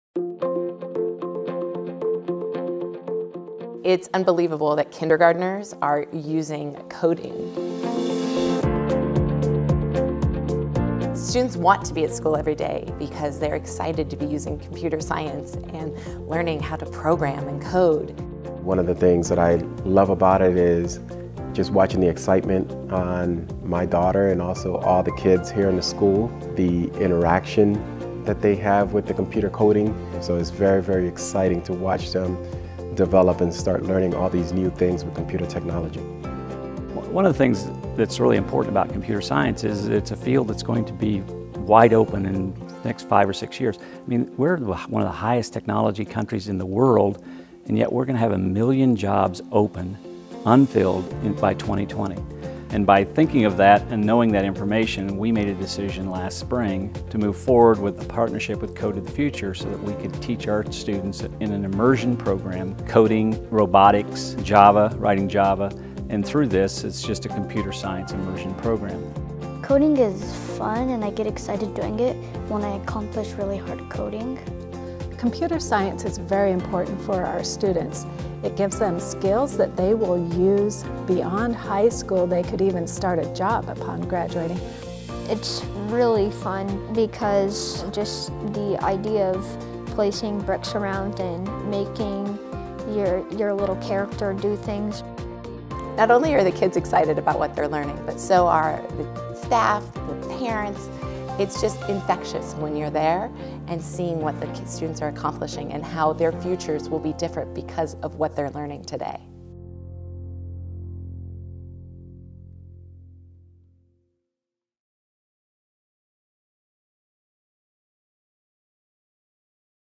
MUSD Computer Science Immersion Schools
Committed to inspiring students to learn today and lead tomorrow, the Marana district is honored to offer the first Code To The Future, America’s first Comprehensive Solution for Computer Science Immersion, Computer Science Immersion Schools in the state!